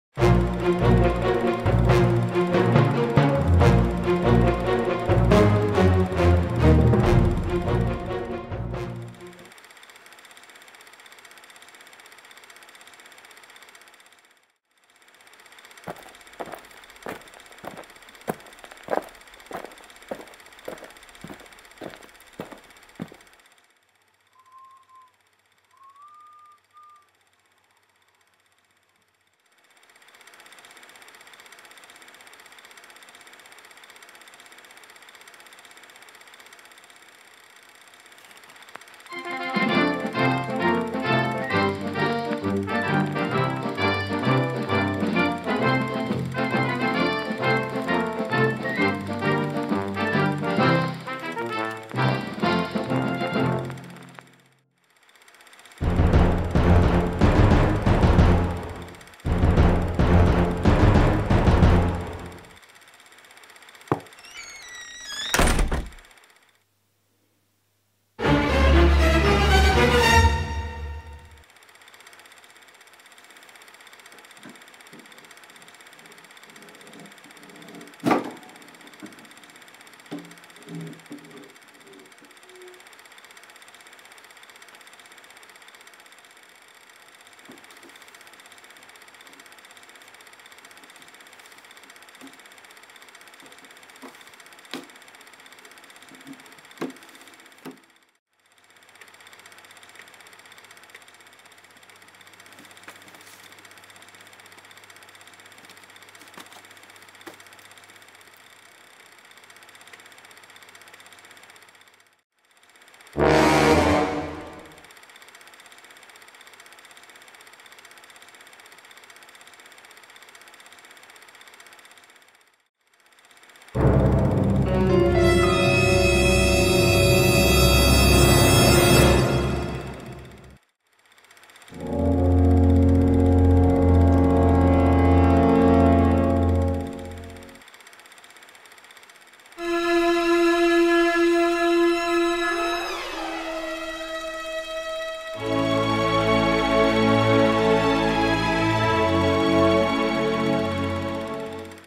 the attic murder.m4v
a short horror film I made entirely using iphone 4 and the app 8mm vintage camera, editing is made in imovie. enjoy